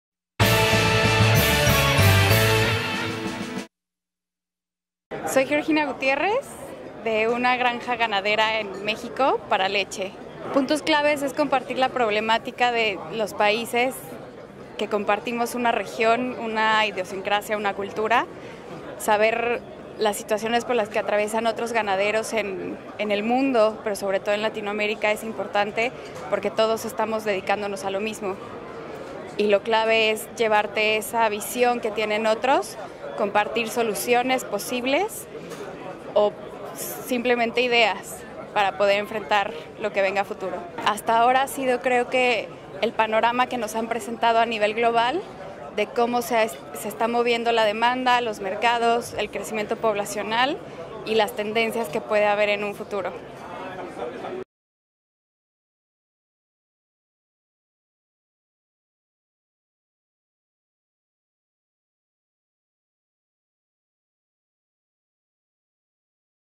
Global Dairy & Beef: Un encuentro para compartir experiencias
Para Georgina Gutierrez, de México, el Global Dairy & Beef es una oportunidade para que los países latinos puedan cambiar ideas con ganaderos de otras partes del mundo.